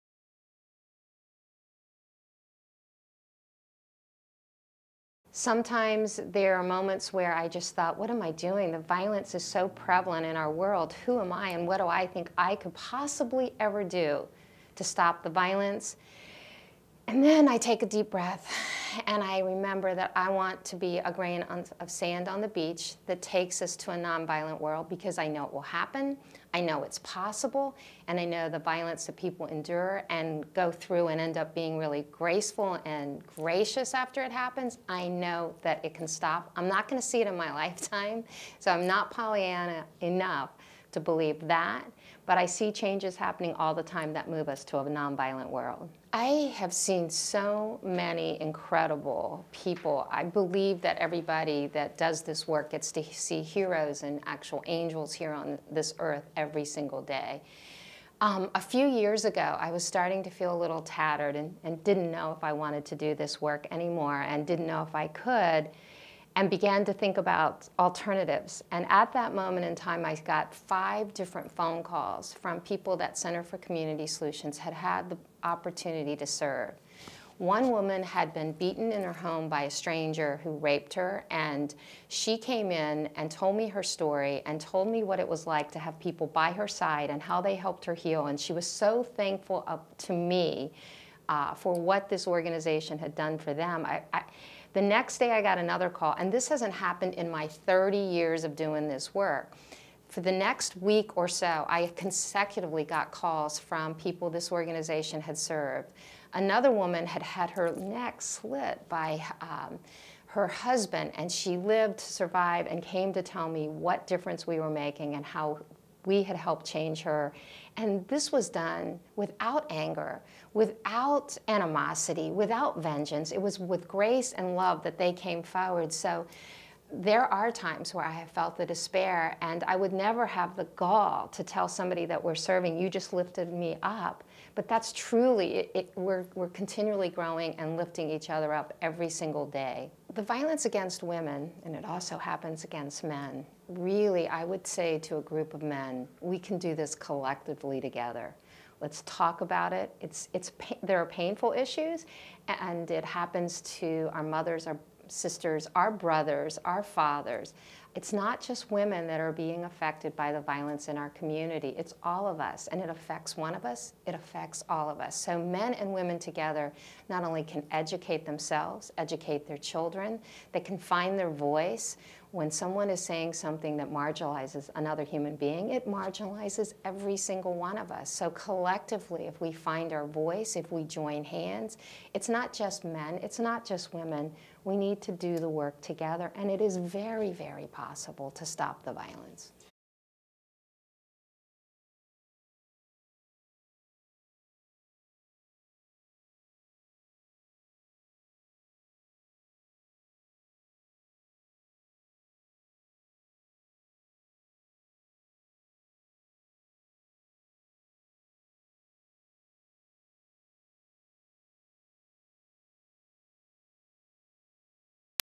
Verna Griffin Tabor -Notes to Our Sons and Daughters My Sister's Voice Exhibiition
I was moved by Malala's story. The power of her message left me wondering: Why do we so often ignore the voices of girls and women?

This project, "My Sister's Voice" provides a space where the voices of women and their invaluable and necessary contributions to our common humanity can be heard. On their faces, we can glimpse the wisdom they've gained from life. From their thoughtful observations, we, all of us, regardless of gender, are invited to a greater understanding of our shared humanity